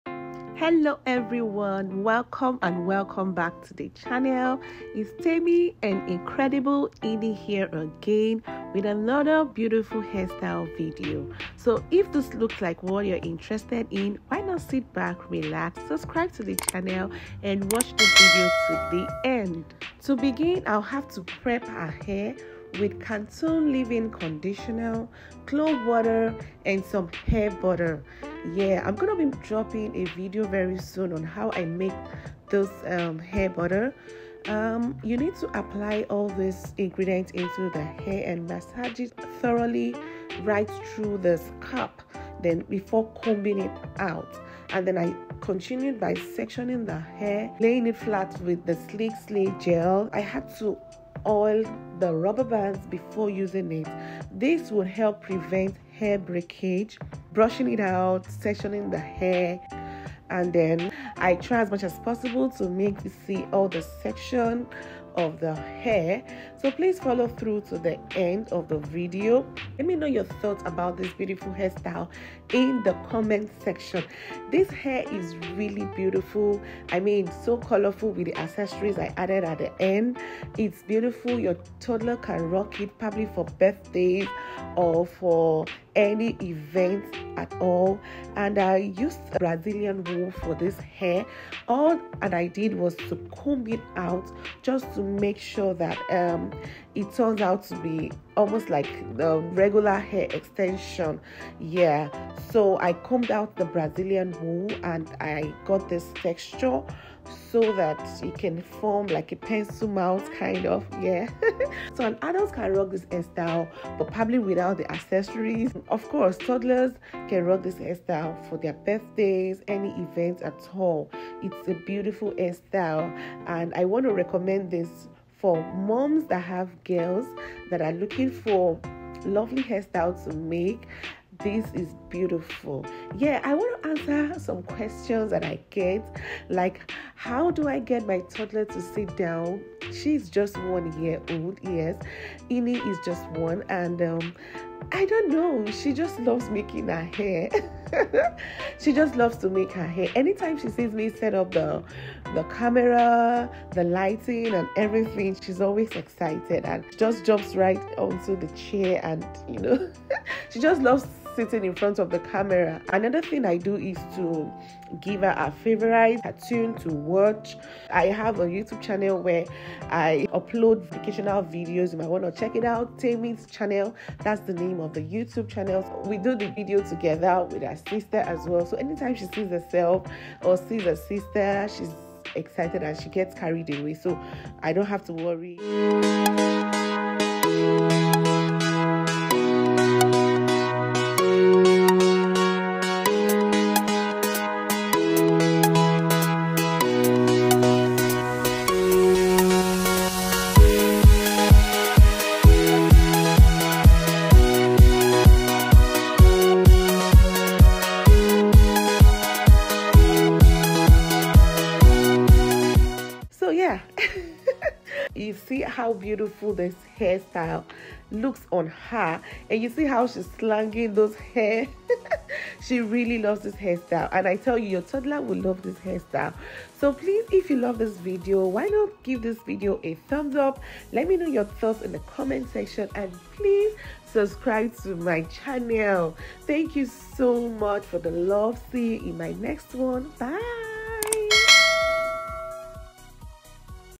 OMG😱 THIS IS THE EASIEST NATURAL LOOKING BRAIDS I'VE EVER MADE ON MY 1YR SHORT HAIR USING WOOL.
Hello Everyone ❤️,

In todays video, I will be showing you how to create this super cute and simple hairstyle on your kids especially for kids/toddler with very short hair and also for moms who can't braid. I hope you have fun recreating it!

Items used;
Cantu leave in condition
Brazilian wool
Combs
Rubber bands
Accessories/Beads
Slick slay edge gel
Hair growth butter

Do go through my channel, you will find videos that will interest you❤️